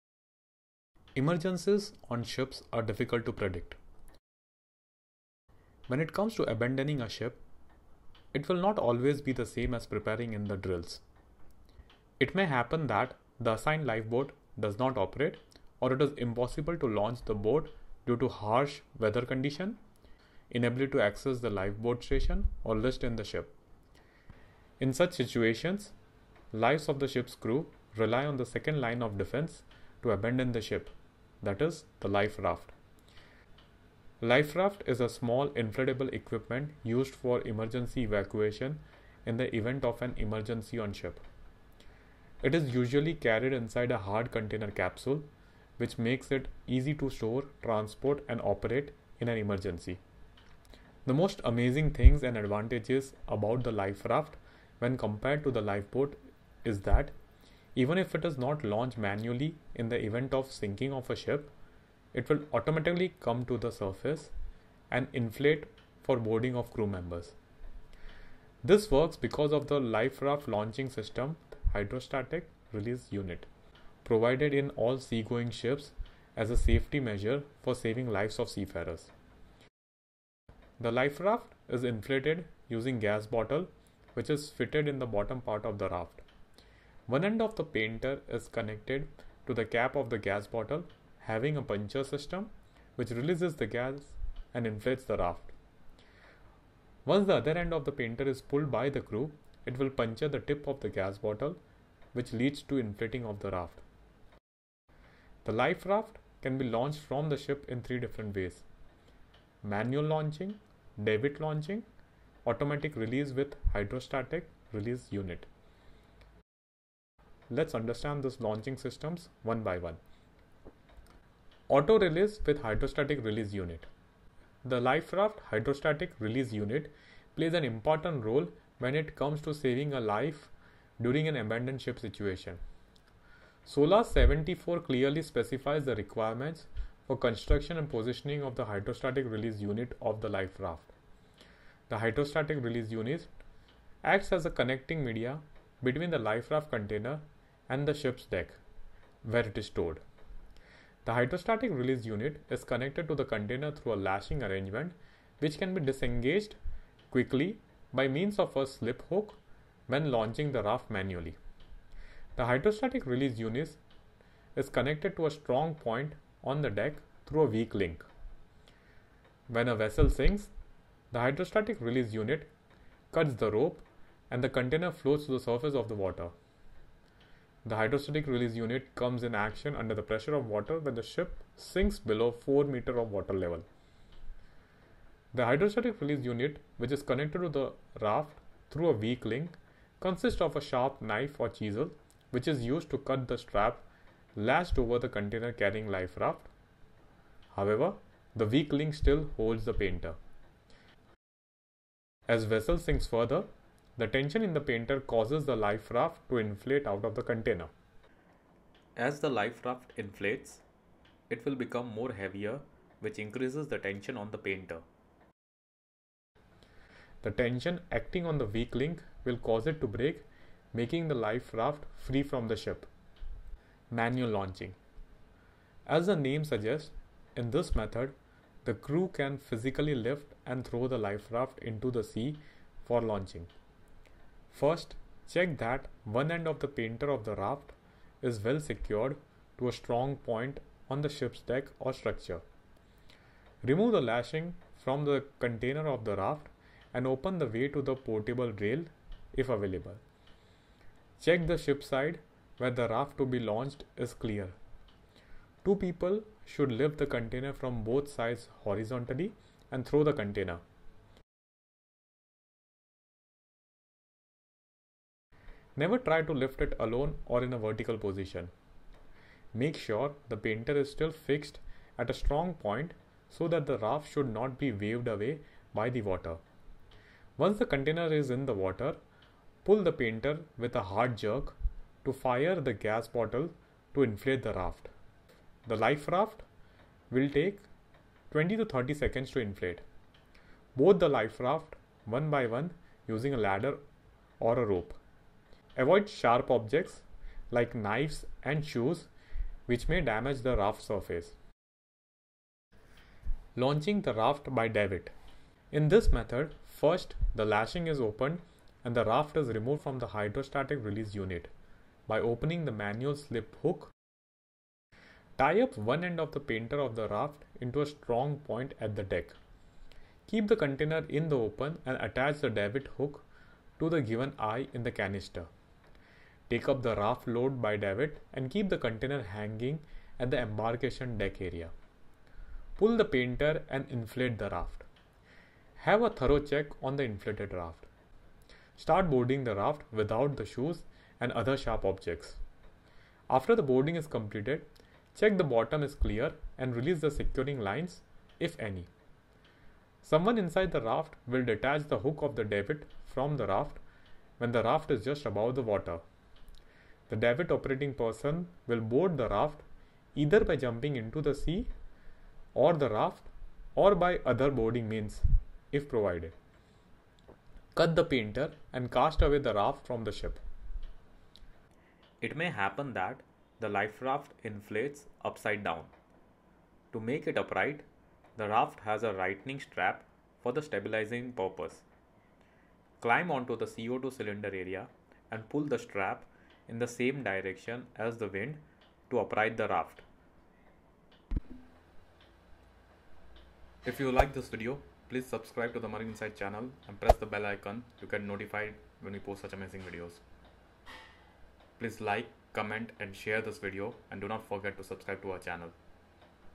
LIFE RAFT LAUNCHING PROCEDURE
Life raft has an advantage over lifeboat as Liferaft launching procedure is easier during emergencies. The life raft inflates itself automatically as it sinks with the ship.

SOLAS chapter III gives all the details for types and the number of life rafts to be carried as per the size and type of the ship.

Life rafts are normally located on muster station, on port and starboard side near the lifeboat, and fwd and aft of the ship. The location generally depends on the size of the ship. Life rafts are stored in a fibreglass container, incorporated with a high-pressure gas used for inflating life raft at the time of emergency. A Hydrostatic Release Unit (HRU) is connected to raft container and ship, which release the raft even after the ship sinks in water.

The particulars of the raft are stencilled on the container which includes the capacity, manufacturing date, servicing date, company name etc. along with the launching procedure with a photogenic display for easy understanding. The basic survival items are already stored in the raft which includes rations, pyrotechnics, life jackets etc.

 In this liferaft launching procedure video, we will see what are the different life raft release systems and launching procedures and how they work.